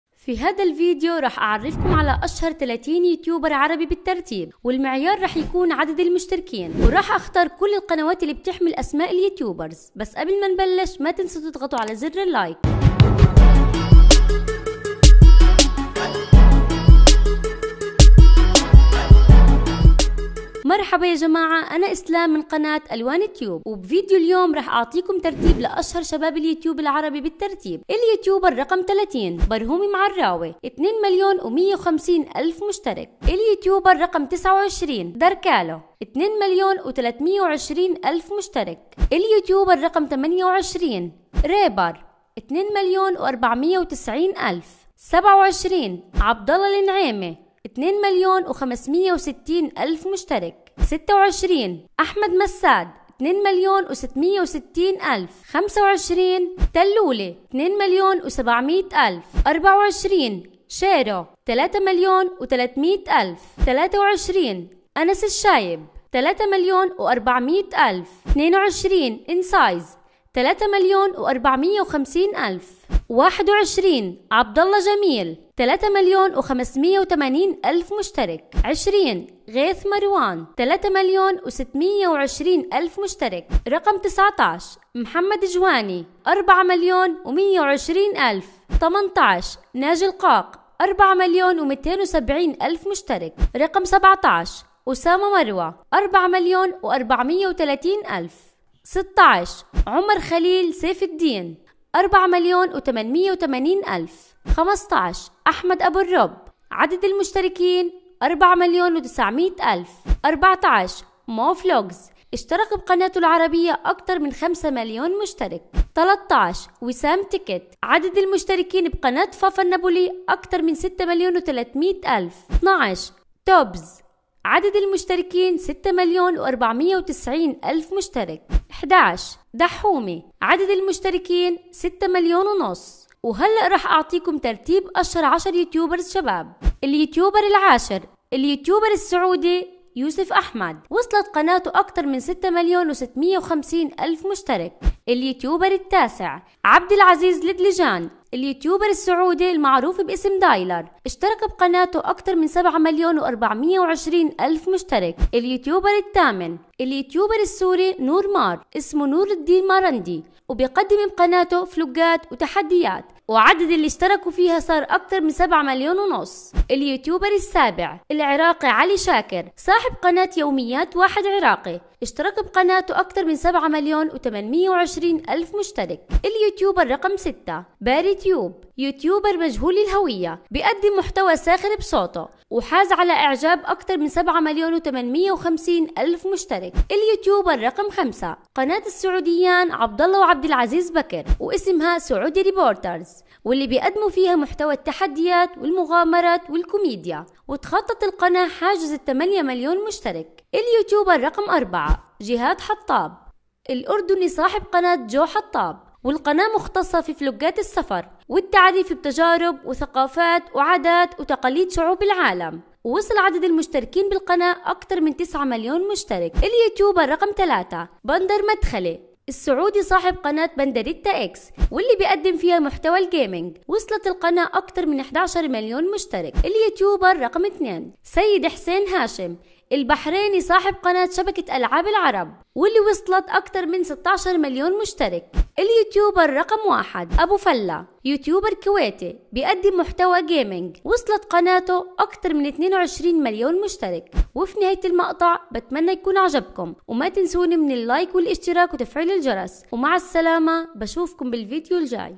أشهر 30 يوتيوبر عربي من الشباب 🏆
أشهر 30 يوتيوبر عربي من الشباب 🏆  تحدثت في هذا المقطع عن : غيث مروان وانس الشايب ومو فلوقز ويوسف احمد وعبد الله النعيمي وفافانابولي واسامة مروة وتوبز ودايلر والعديد من اليوتيوبرز العرب الشباب وما عليك سوى الانتساب إلى هذه القناة للاستفادة من المزايا: